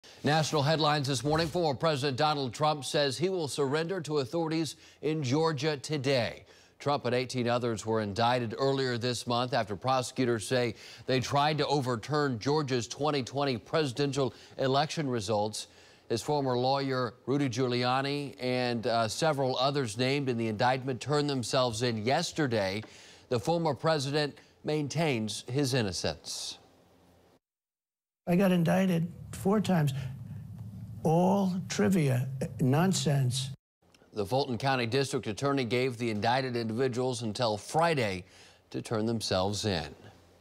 Trump Set To Surrender At Georgia Jail On Charges That He Sought To Overturn 2020 Election
Former President Donald Trump is expected to surrender to authorities on Thursday at the Fulton County Jail in Georgia.